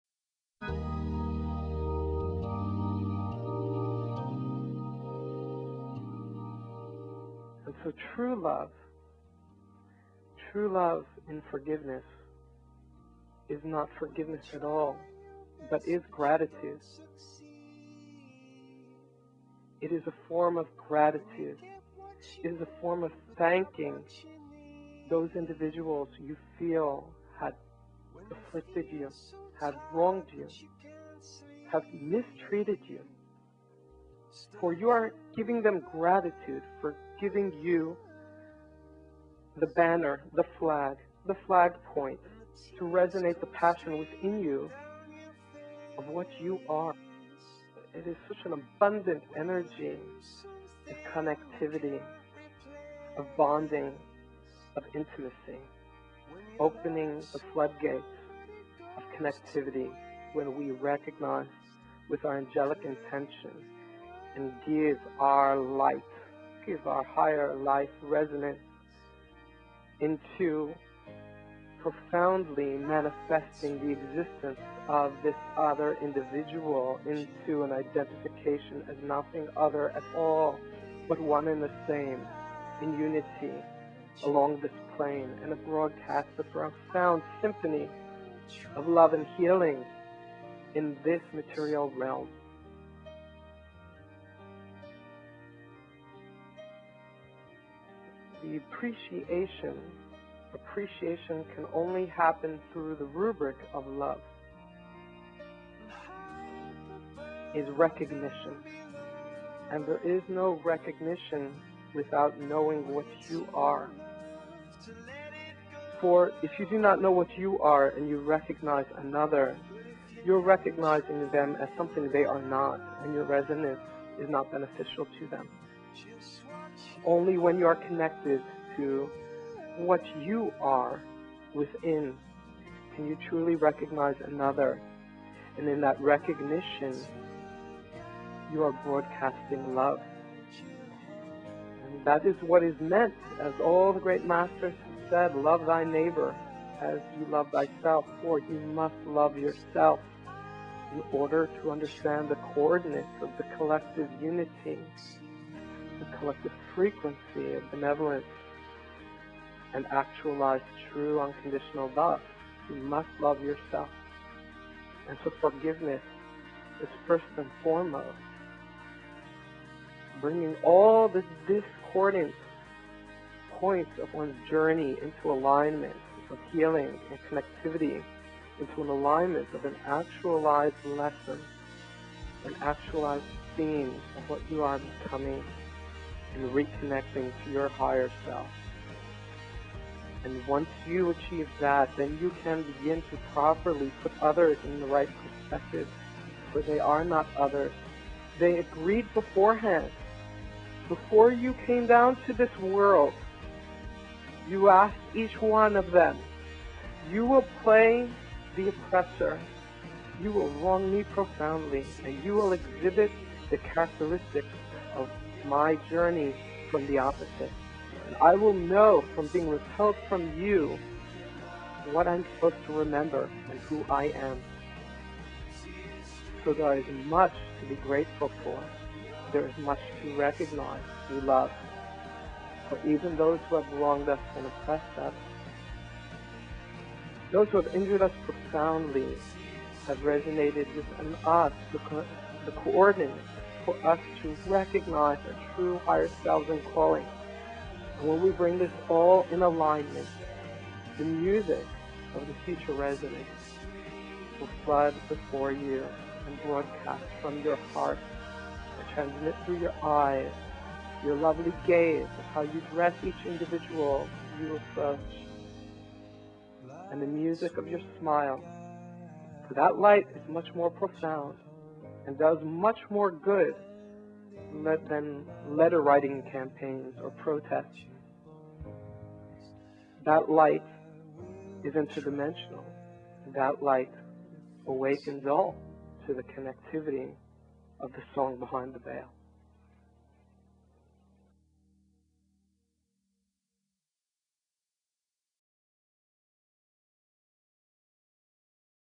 Forgiveness + A Higher Love = Gratitude
Many people claim to forgive, but what they truly mean is that they learn to forget... In truth, TRUE FORGIVENESS is "GRATITUDE". Coming to an acknowledgment of thanks for those that taught you lessons from "within" or have oppressed you in to knowing who you are through lessons of "without". Protests and reprisals are often futile, instead the negative momentum of energies must be inverted at their most abstract root in Spirit, and then healing can begin.